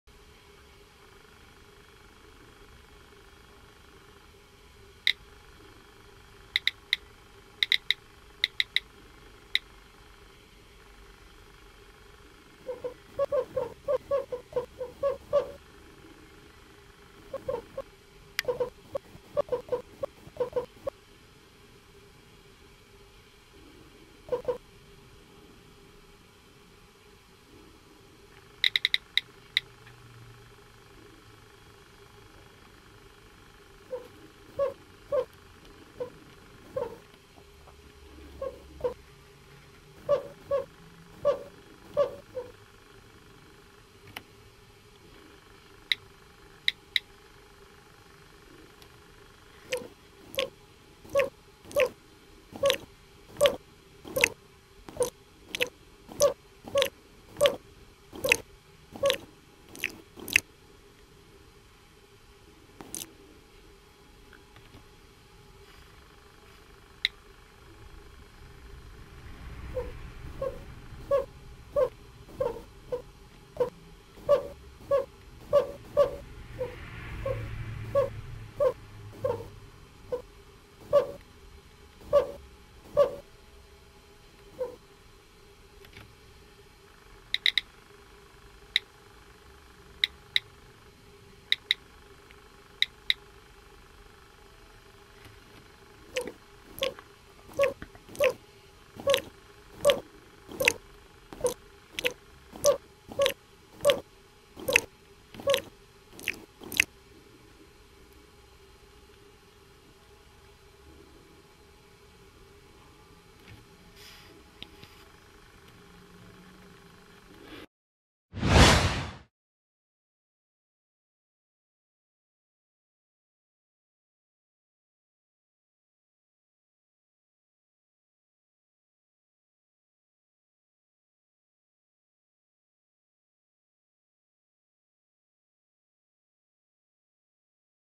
iPod and iPhone screen cleaning
SlurpsClean is the new app to clean your dirty iPhone and iPod touch screen.